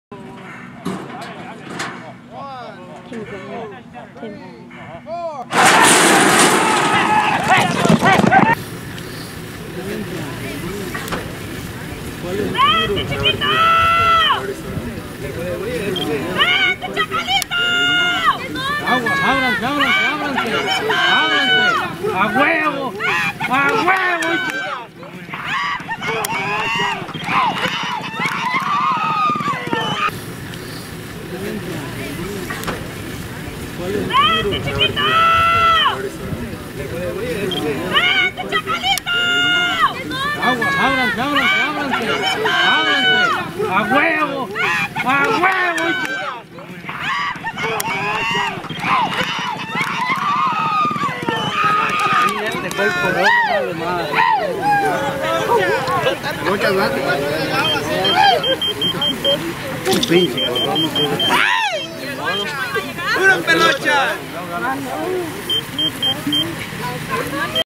El Chacalito vs Bojangles vs Romeo
M&M Downs 16 de Septiembre 2018
El Chacalito Cuadra Pelones Jimenez Jockey Pelochas
vs
Bojangles Owner Scooter Hall Jockey Timmy
vs
Romeo Owner M&M Jockey Salgado